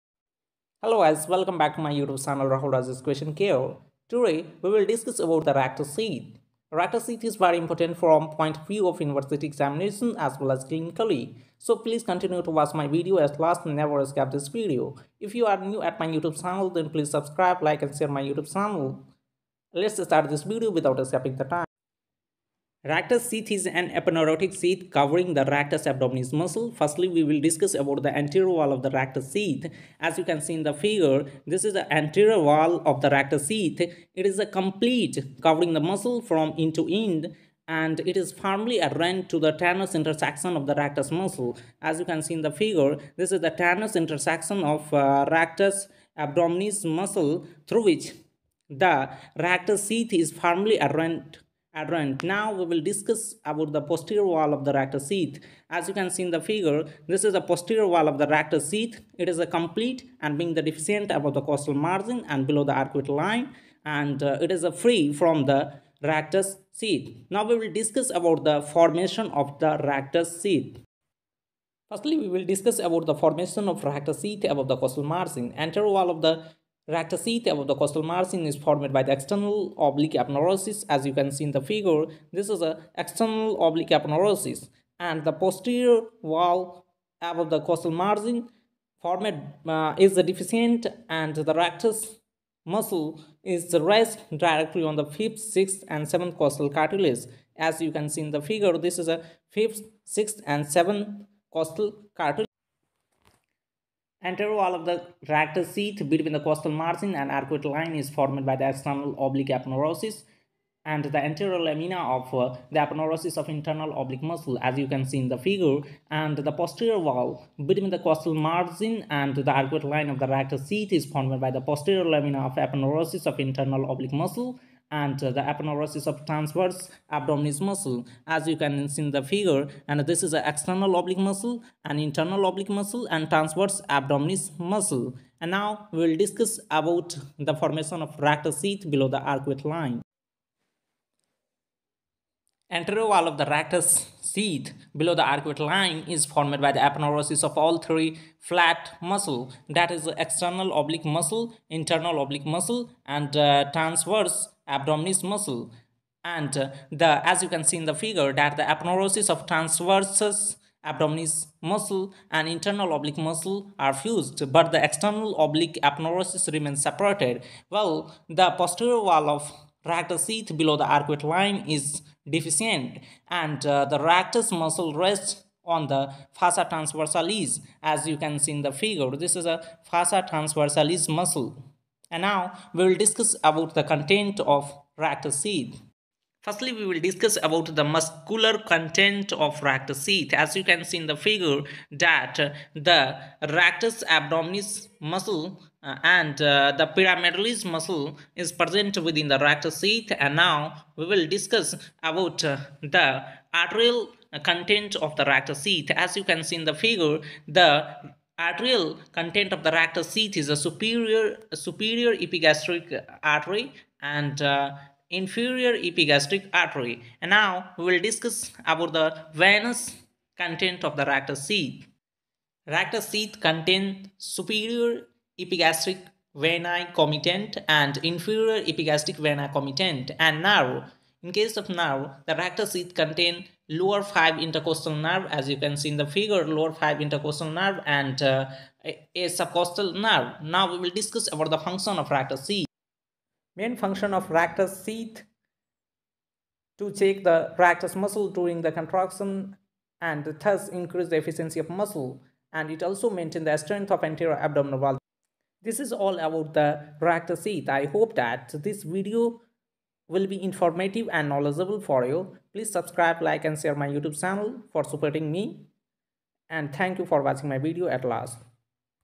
Popliteal fossa/Boundary of Popliteal fossa/Roof, floor, Content of Popliteal fossa
Quiery word-
this video is prepaired by Dr.Rahul kumar..this video is very informative for mbbs student and this video is very knowledgeable